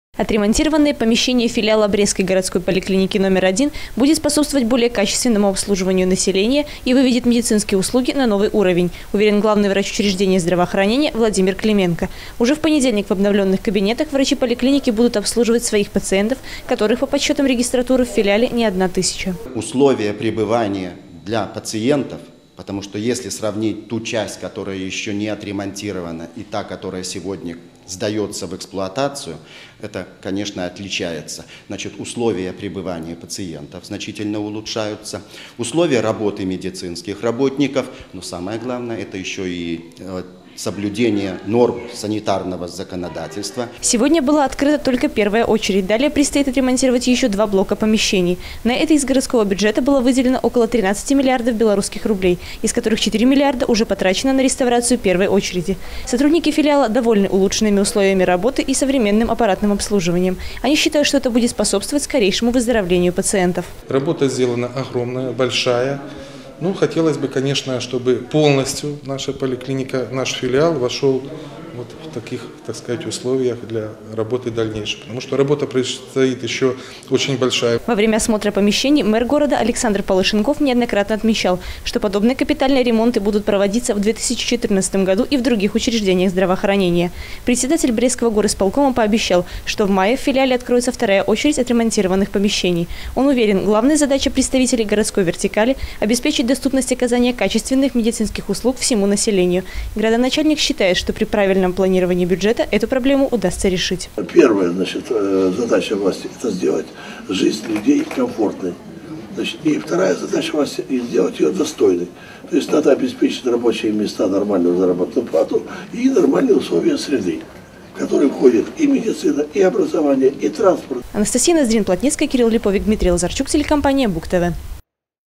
2014-03-07 г. Брест Телекомпания "Буг-ТВ". Открытие после реконструкции филиала поликлиники №1 ЦГП.
Открытие после реконструкции первой очереди  филиала  поликлиники №1 ЦГП.
Работа системы здравоохранения Брестской области направлена, в первую очередь, на сохранение и укрепление здоровья населения региона. Для этого в области реализуется комплекс мероприятий, ориентированных  на создание оптимальных условий для оказания качественной медицинской помощи, а также  на модернизацию и техническое оснащение  поликлиник и больниц.
Сегодня после реконструкции открылась первая очередь филиала №1 первой Брестской городской поликлиники. На торжественном открытии присутствовал мэр города Александр Палышенков. Градоначальник лично оценил степень готовности учреждения к приёму больных.